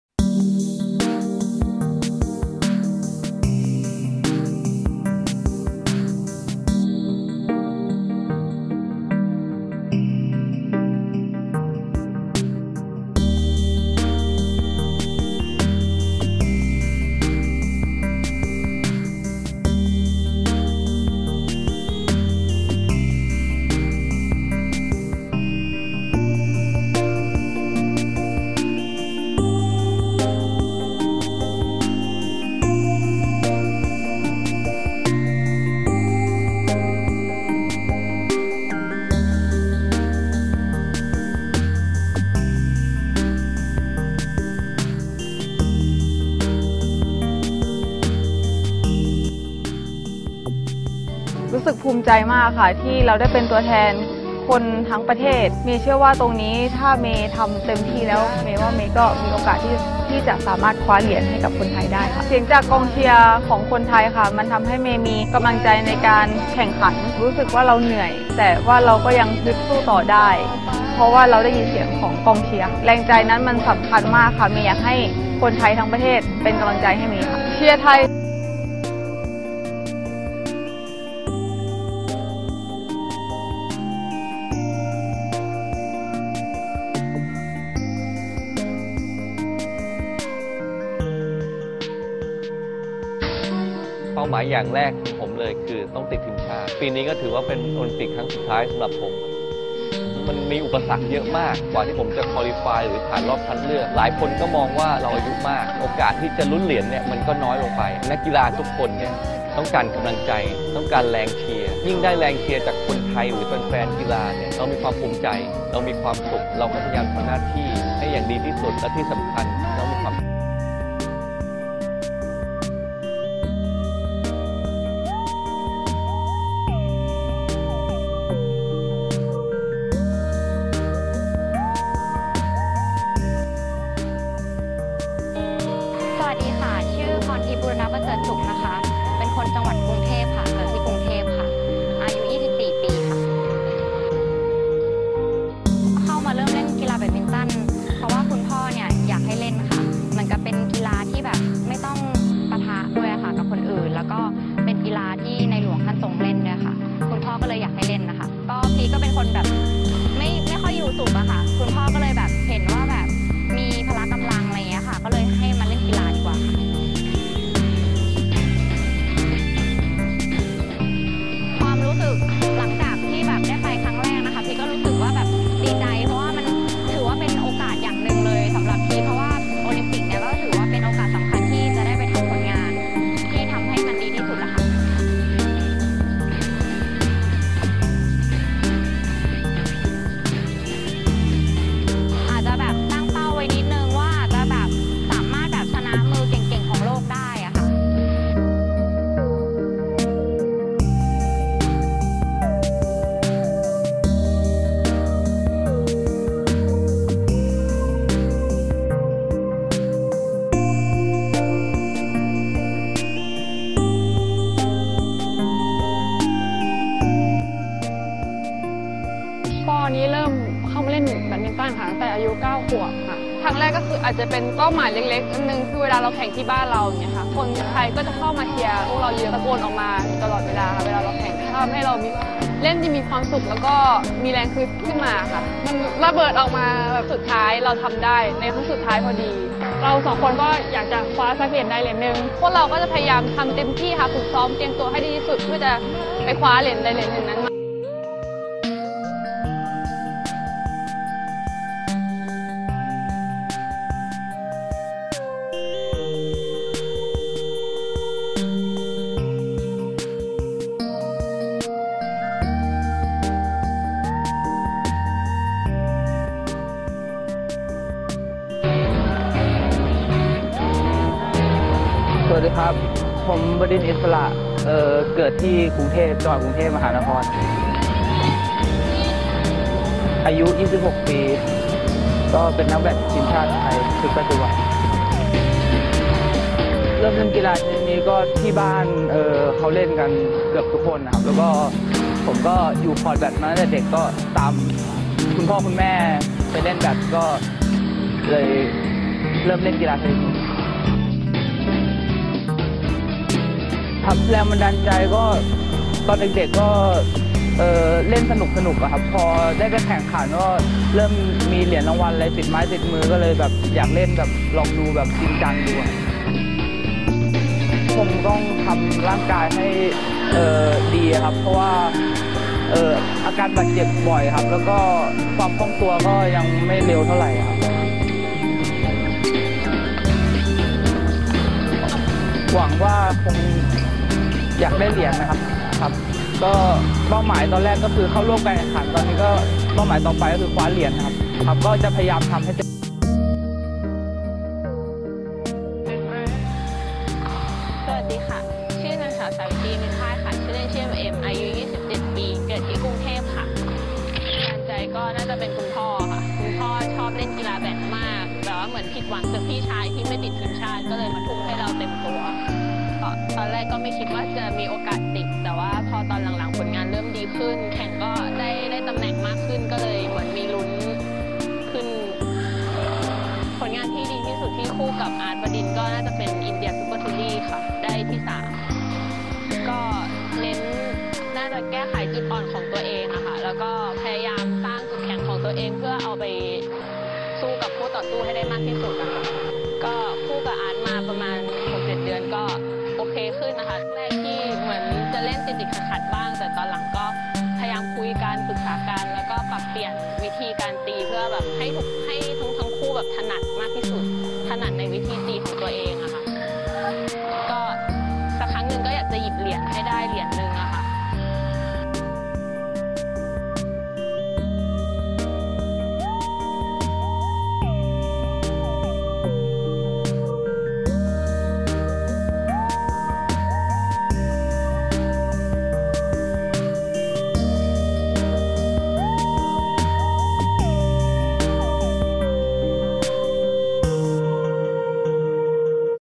ร่วมเชียร์..7 นักแบดมินตันไทย โอลิมปิกเกมส์ 2016
รอบแบ่งกลุ่ม วันที่ 11-14 สิงหาคม 59 เวลาไทย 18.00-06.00 น.
ระหว่างวันที่ 11-20 ส.ค.2559 ที่นครริโอ เดจาเนโร ประเทศบราซิล โอลิมปิกเกมส์ ครั้งที่ 31